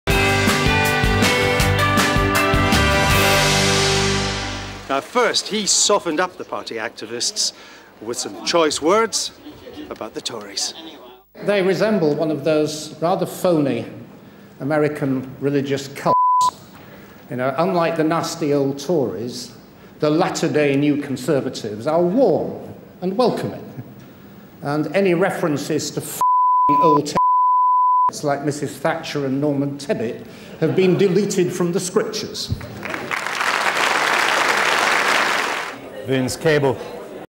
Lib Dem Outburst 2009 Conference
UK MP and Lib Dem hero Vince Cable tells it like it is...